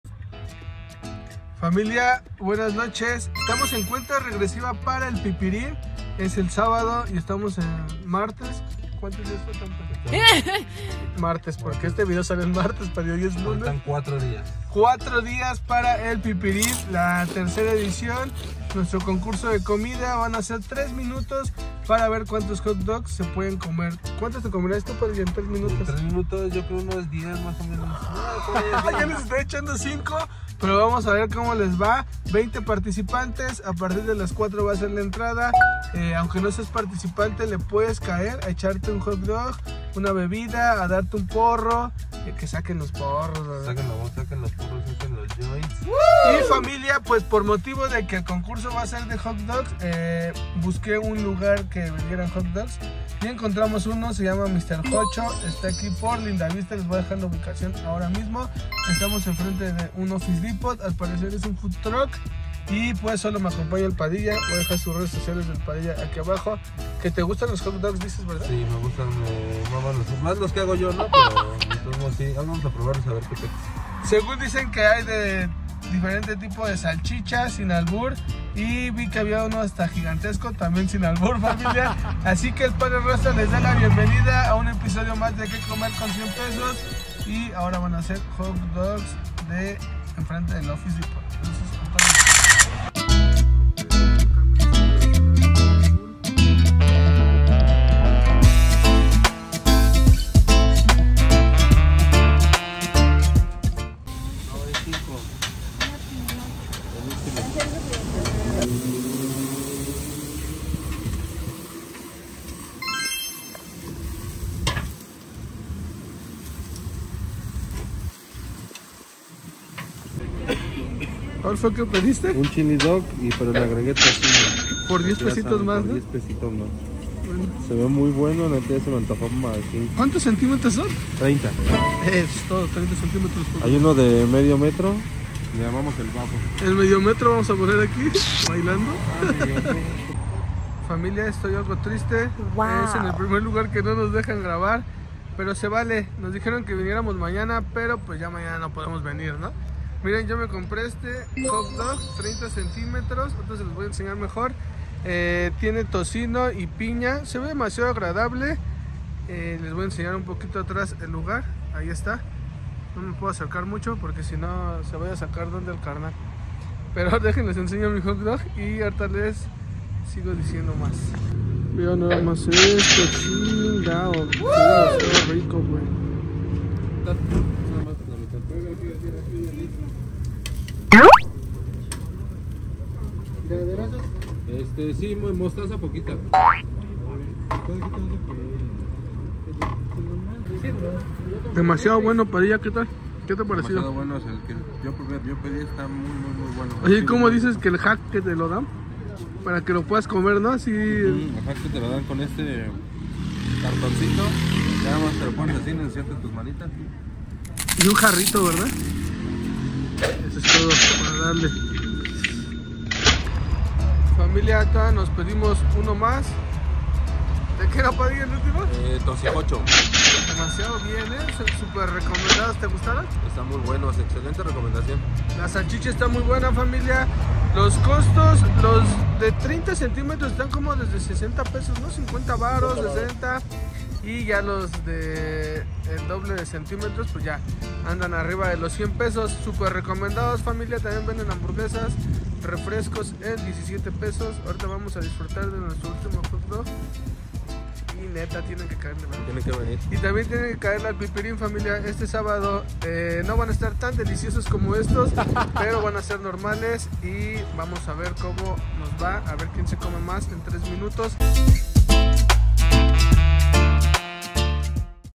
¿Que comer con $100?. Ep 42 "Comiendo Hot Dogs gigantes en Mr. Hochos”
Familia  estamos en cuenta regresiva para nuestro concurso de comida, el pipirin, 20 participantes, 3 minutos, cuantos hotdogs te comerías?

Por ese motivo nos lanzamos a buscar hotdogs gigantes y nos encontramos con Mr Hochos, ubicado en un foodtruck en avenida politenico en la delegación gam, hay de 30 y 60 cms de todo tipo con Piña, tocino y hasta chillidog.

Costos accesibles desde $50 pesos y refrescos $17